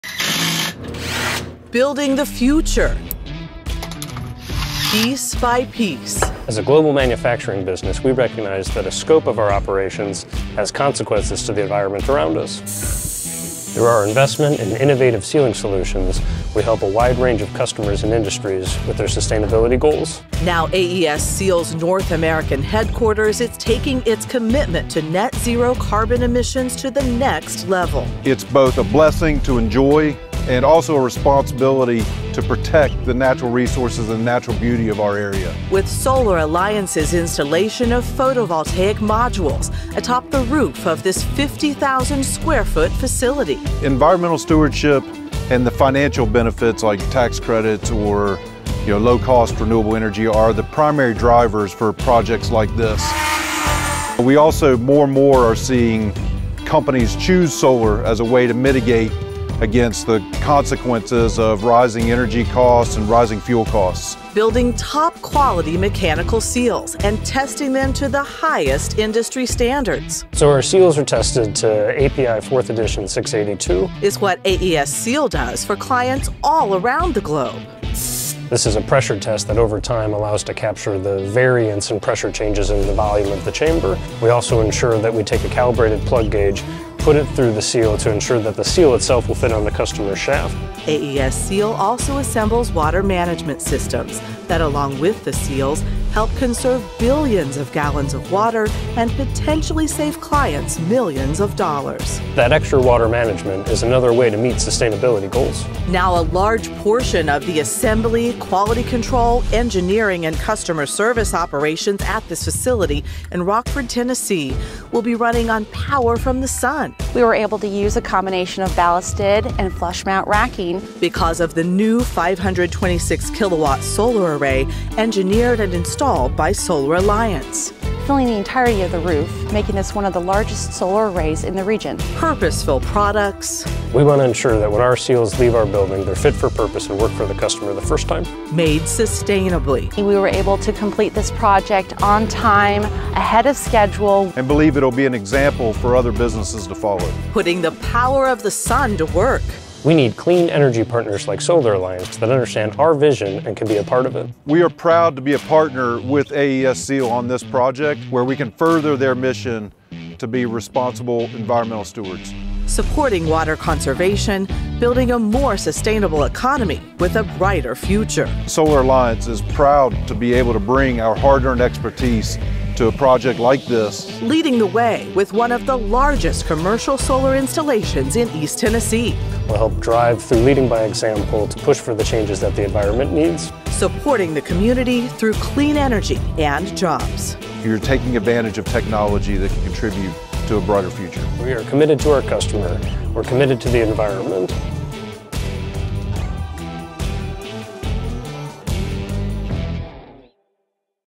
Solar Alliance Supporting AESSEAL, Inc. Mission with New Solar Array - Full Story
AESSEAL, Inc. is leading the way with one of the largest commercial solar installations in East Tennessee. This is a 526-kilowatt array designed, engineered and installed by Solar Alliance on AESSEAL, Inc.'s North American Headquarters. It utilizes two different forms of racking atop the 50,000 square foot facility.

This installation supports the leading mechanical seal manufacturer's Net Zero environmental efforts. AESSEAL, Inc. Operations Manager Chris Staackmann said, "As a global manufacturing business, we recognize that a scope of our operations has consequences for the environment around us."

"We were able to complete this project on time, ahead of schedule," said Solar Alliance Commercial Project Estimator Alex Mas.

"And believe it will be an example for other businesses to follow," said Solar Alliance General Manager Jon Hamilton.

AESSEAL, Inc. Operations Manager Chris Staackmann said, "As a global manufacturing business, we recognize that a scope of our operations has consequences for the environment around us."